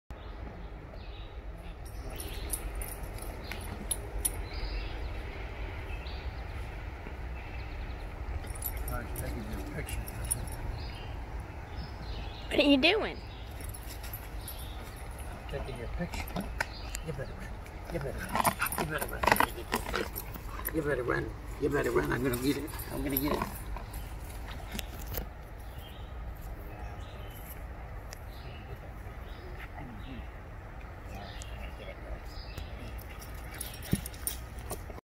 Nelson The Skye Terrier Frisbee
Teaching Papa how to play loser frisbee